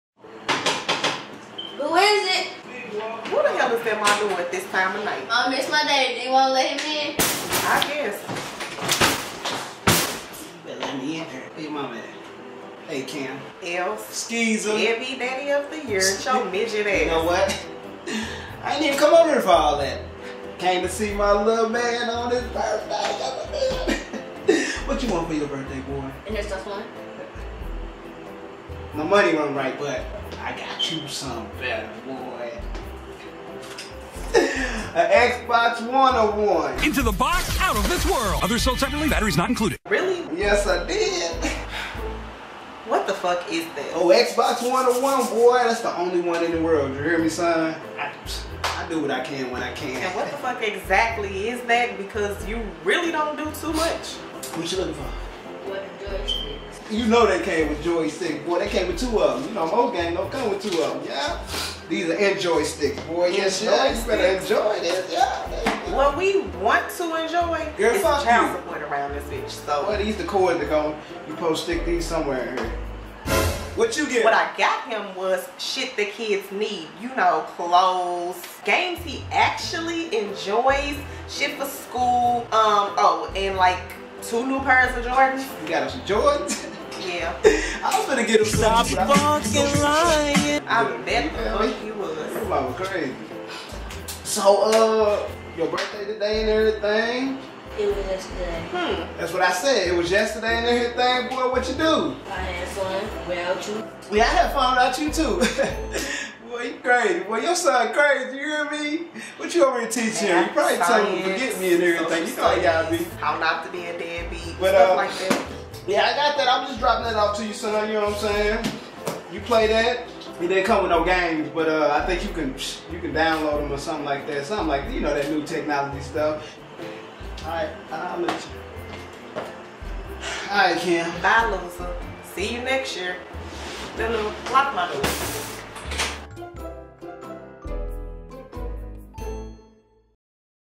Birthday Boy
I just wonna be civil with the BM around the kids. idk y'all watch and tell me am I wrong for trying! Errtime I look up, its one of my kids birthday!